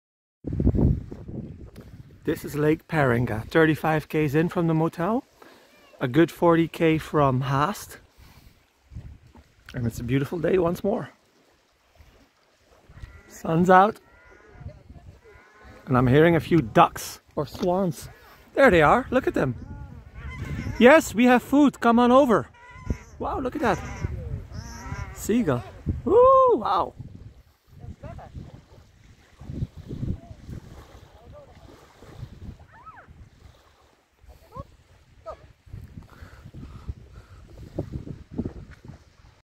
Lake Paringa and two low flying birds
A gorgeous view of Lake Paringa, some 40 km from Haast.